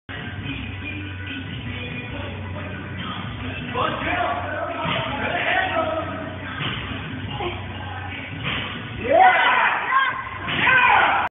16.3 Rx Rookie Style
Most disgusting muscle up but it counts! 3 months in and got 2!